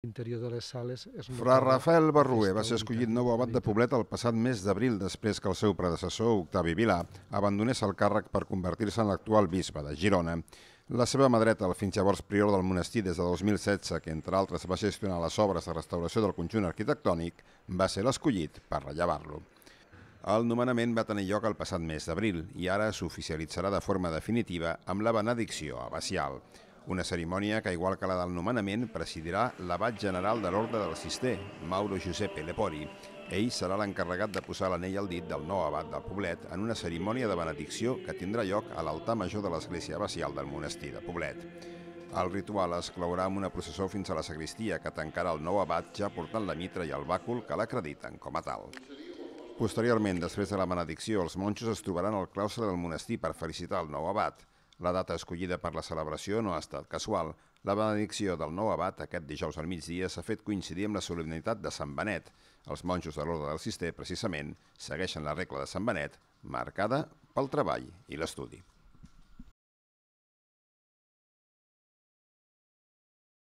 Tot a punt per a la benedicció del nou abat de Poblet
Rafel Barrué serà de totes totes demà abat de Poblet. Ja va ser nomenat com a successor de l'actual bisbe de Girona i demà rebrà la benedicció abacial de mans de l’abat general de l’Orde Cistercenc.